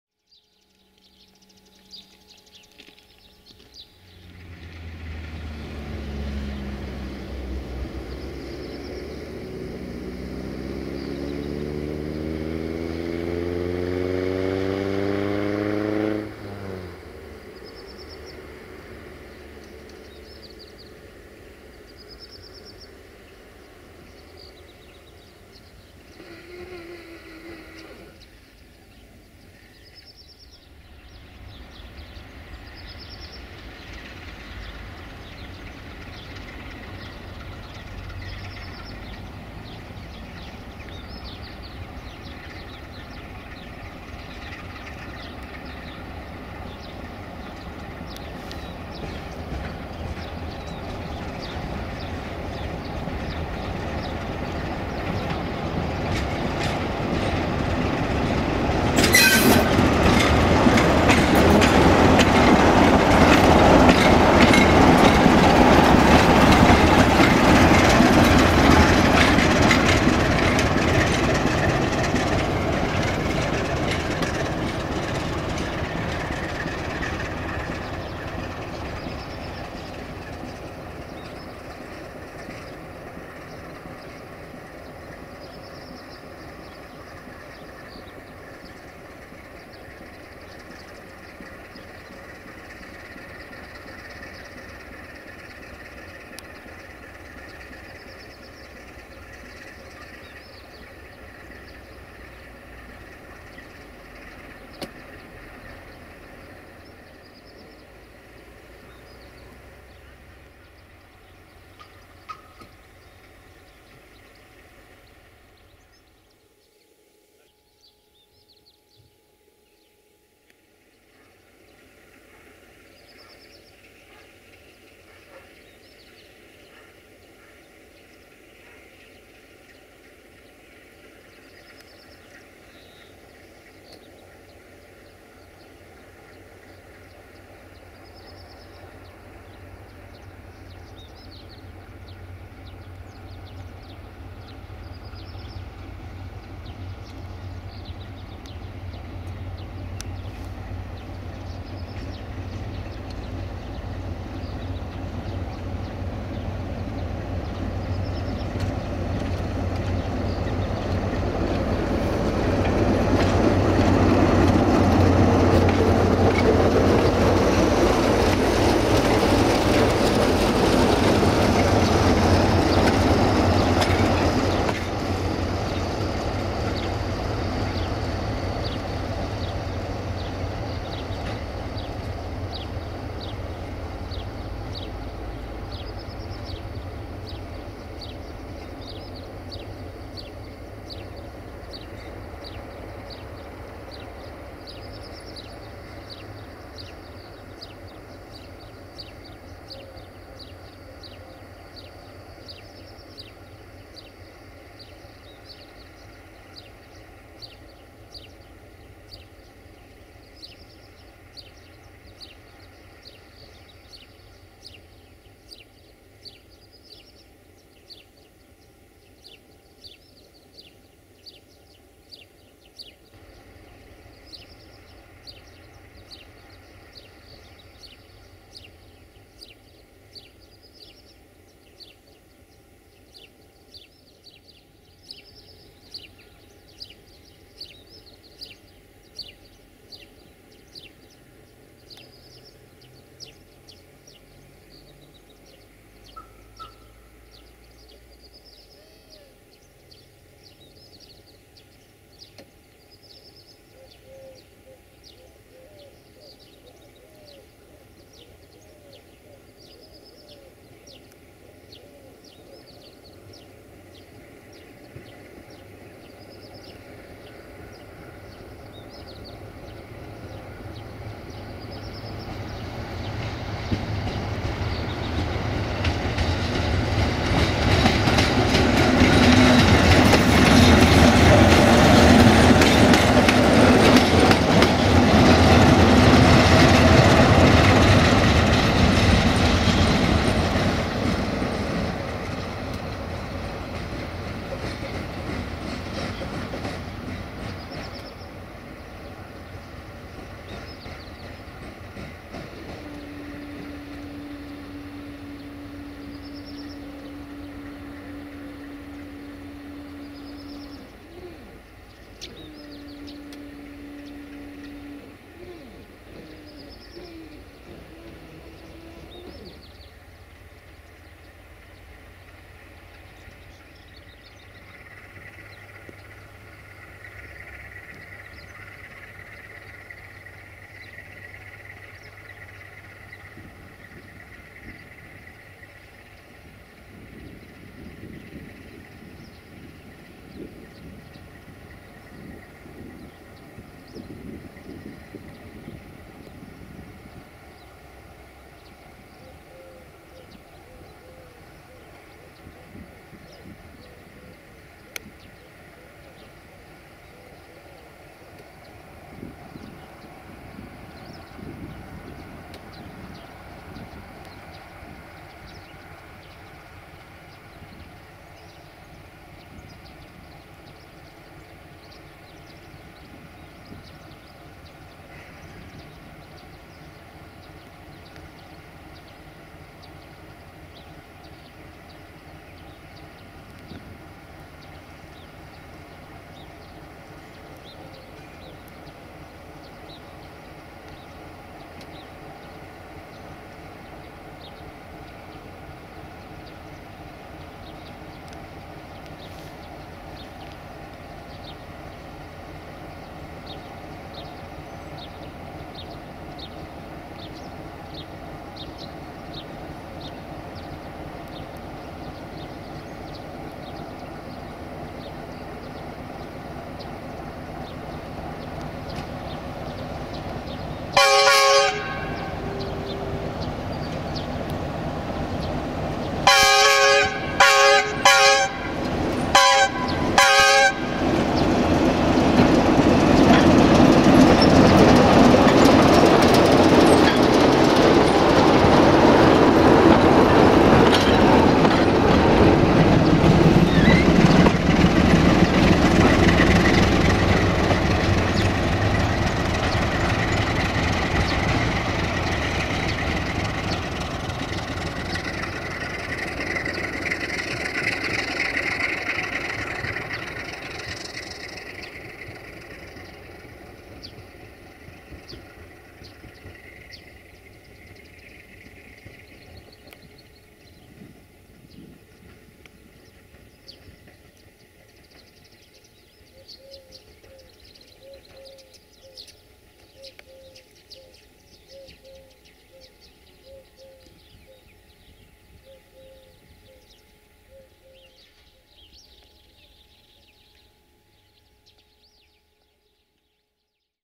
Fluieroasa 60-0714-5&60-1151-9 Whistle Loko & LDE2100 60-0434-0 in Gara Aleșd Station 07 April 2019
1:Manevra/Shunting - Fluieroasa/Whistle 060-DA/LDE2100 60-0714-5 RO-VTR
2:Manevra/Shunting - Fluieroasa/Whistle 060-DA/LDE2100 Loko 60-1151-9 RO-SNTFM
3:Prva Slovenska Zeleznicna - 060-DA/LDE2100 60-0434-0 PSZ/RO-BEU

07 April 2019